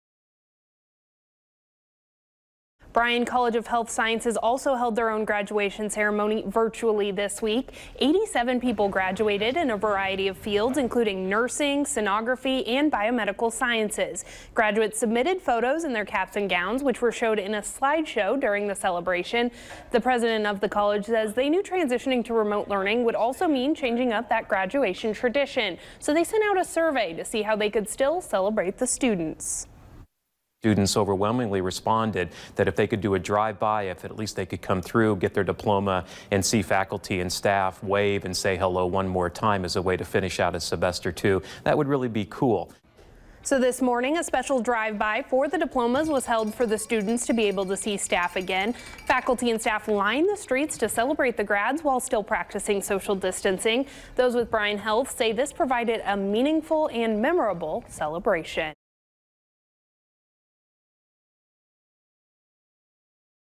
10/11 NOW: Bryan College Honors Graduates with Virtual Commencement, Diploma Pick-Up
Bryan College of Health Sciences also held their own graduation ceremony virtually this week.

87 people graduated in a variety of fields including nursing, sonography and biomedical sciences. The president of the college said they knew transitioning to remote learning would also mean changing up the graduation tradition.

Saturday morning a special drive by for diplomas was held for the students to be able to see the staff again. Faculty and staff lined the streets to celebrate the grads while practicing social distancing. Those with Bryan Health said this provided a meaningful and memorable celebration.

10/11 NOW anchor, Mackenzie Huck, has the story!